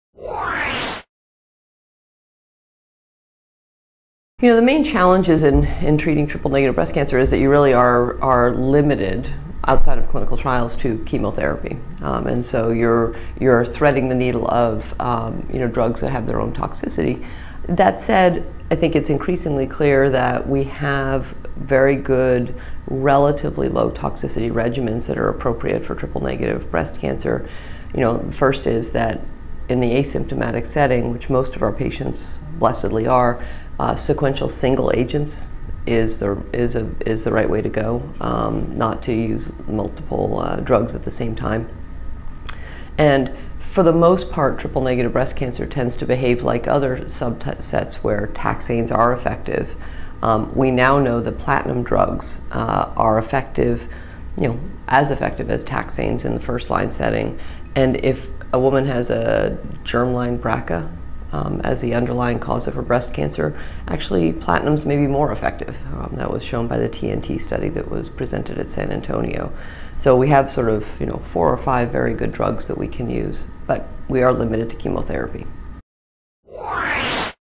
Dr. Carey on Treating Triple-Negative Breast Cancer
Lisa A. Carey, MD, professor of medicine, Breast Cancer Research, University of North Carolina, medical director, UNC Breast Center, chief of Hematology/Oncology, physician-in-chief, UNC North Carolina Cancer Hospital, discusses treatment options and challenges in triple-negative breast cancer.